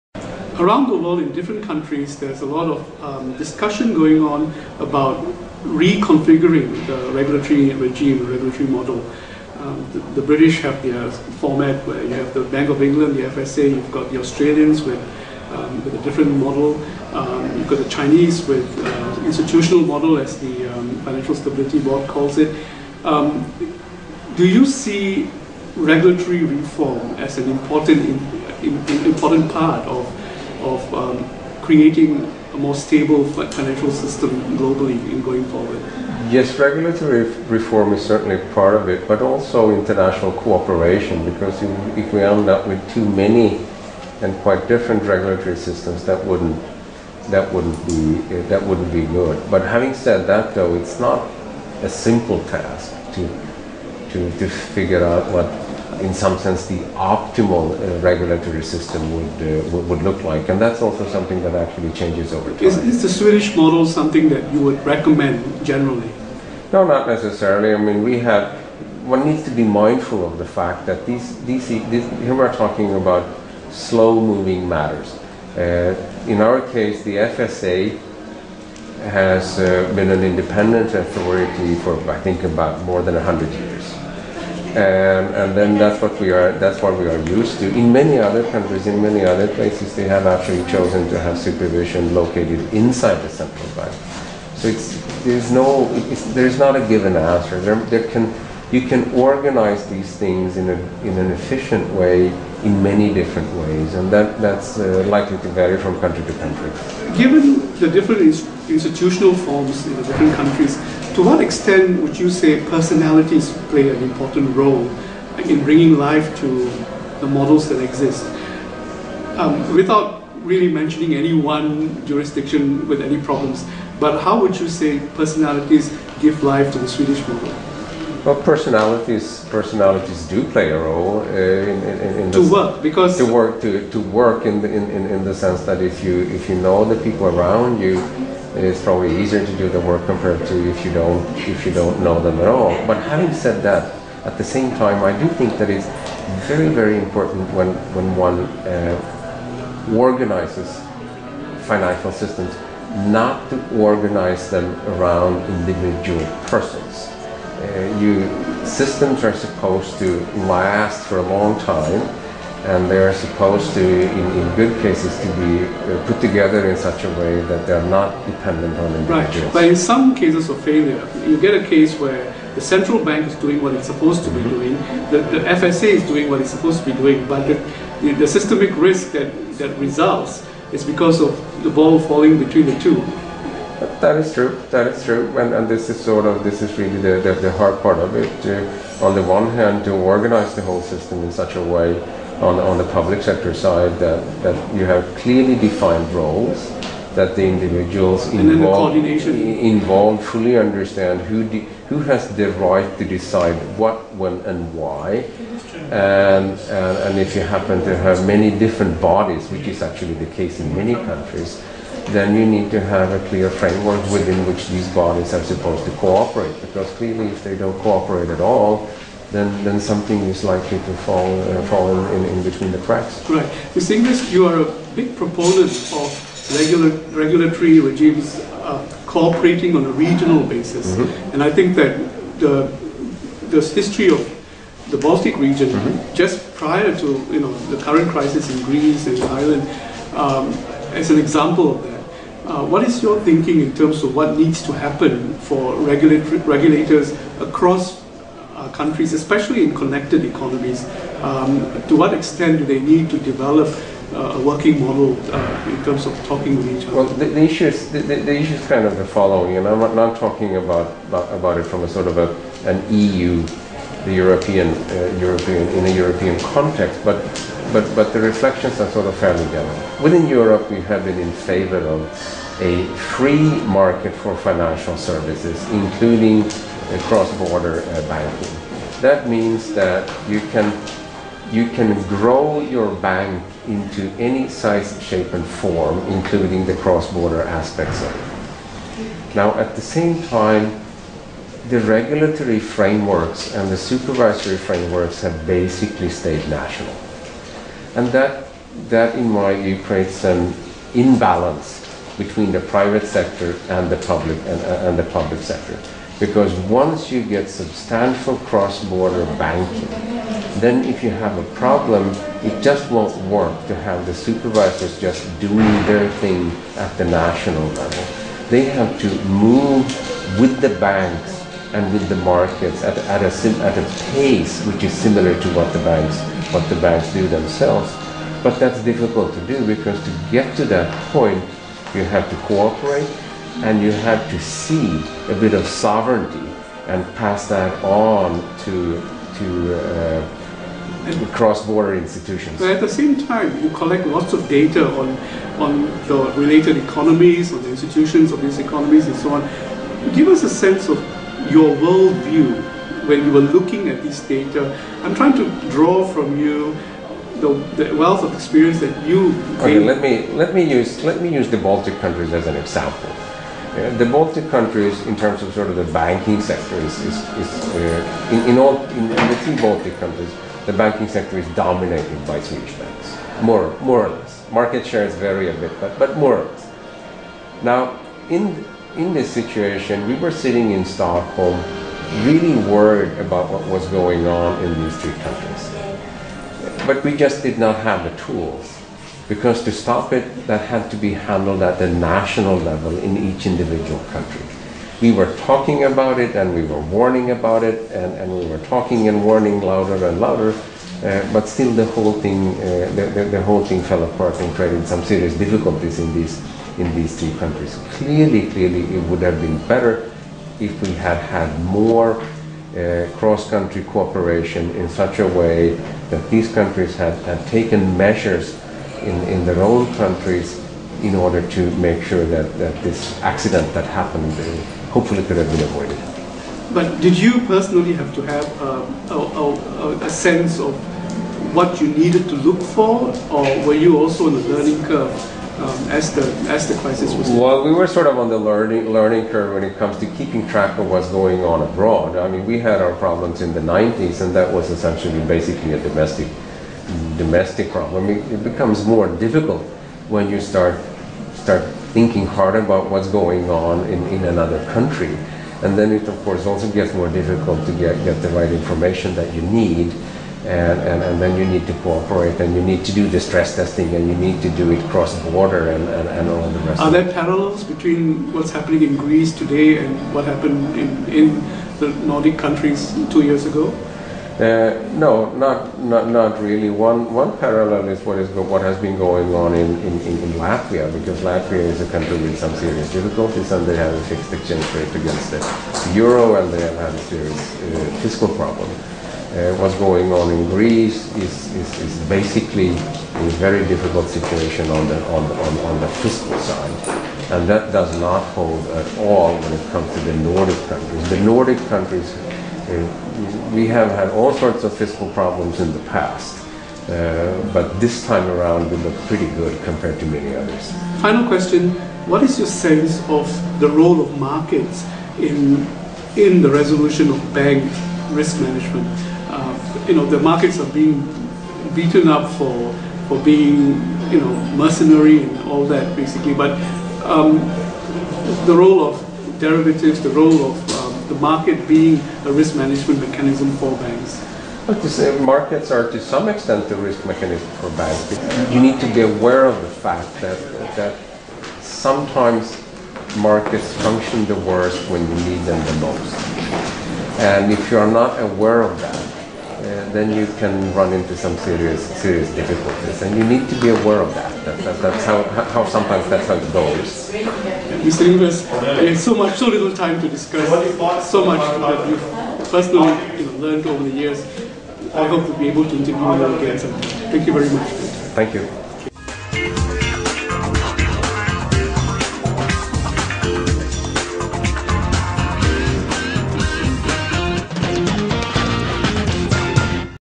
"Sweden's lessons from the Latvian crisis of 2008"
(Part 2 of 2) Conversation with Stefan Ingves, chairman of the executive board and governor of the Riksbank, Swedens central bank, on lessons from the Latvian crisis  the reconfiguration of the regulatory regime  regional co-operation between regulators  the role of markets in banks risk management